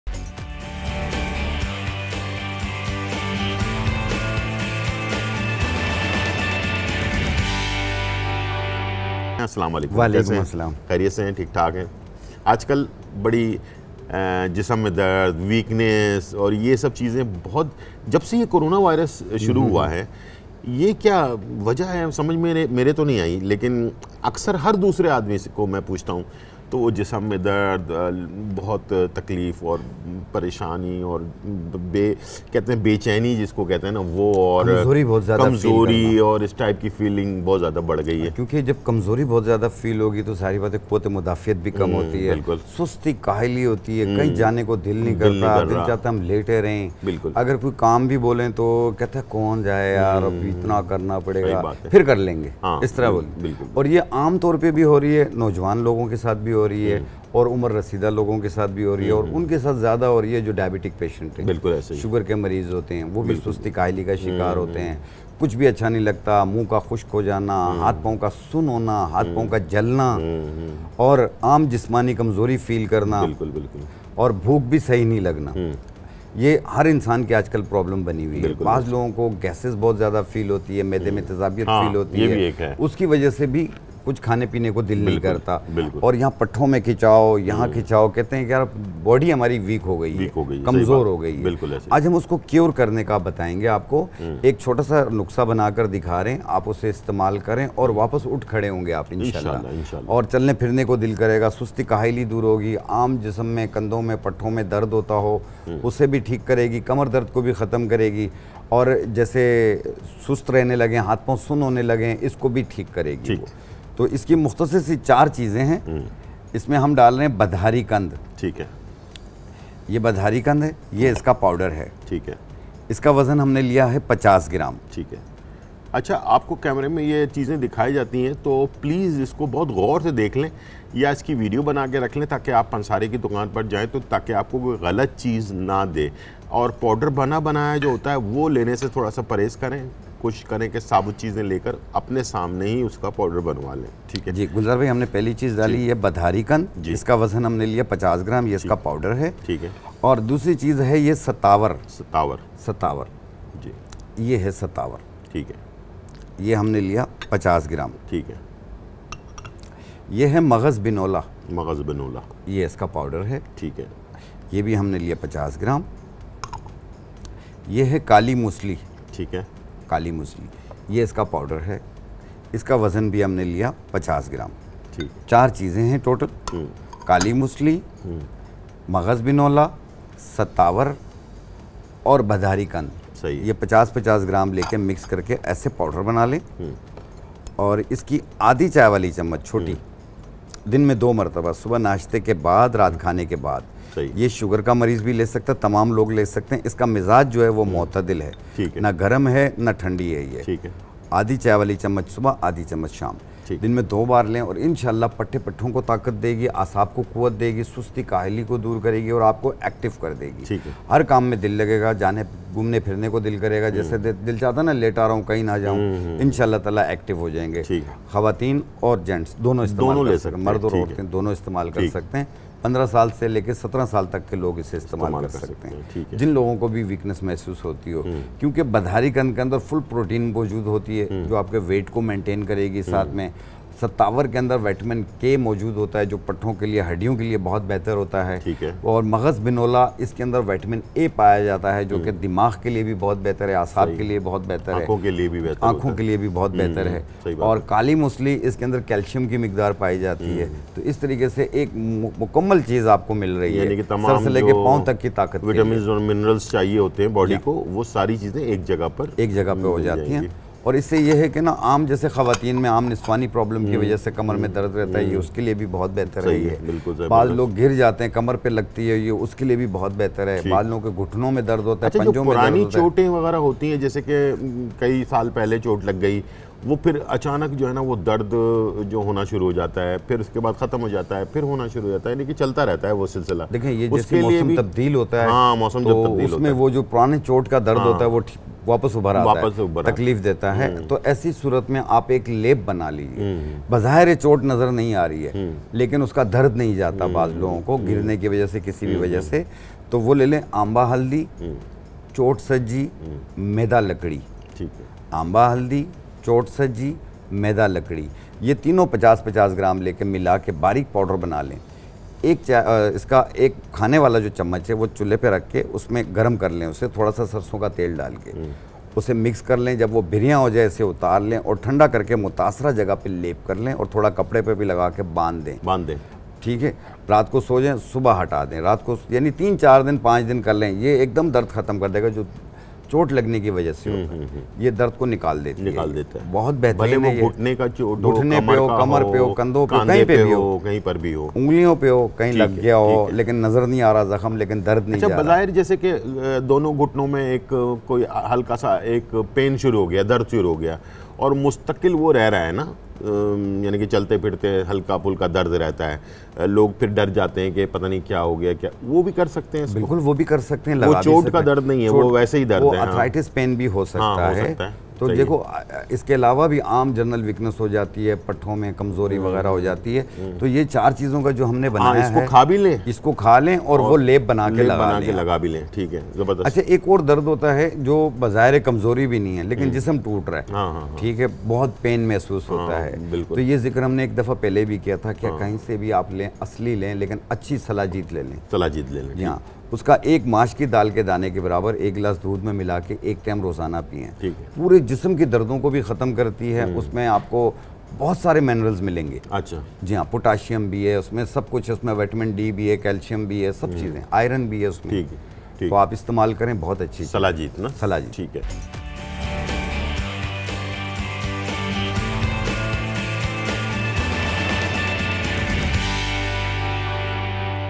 Joron Kay Dard Aur Kamzori Ka Behtreen Nuskha | Aaj Ka Totka by Chef Gulzar
Chef Gulzar brings the best Home remedies for his fans.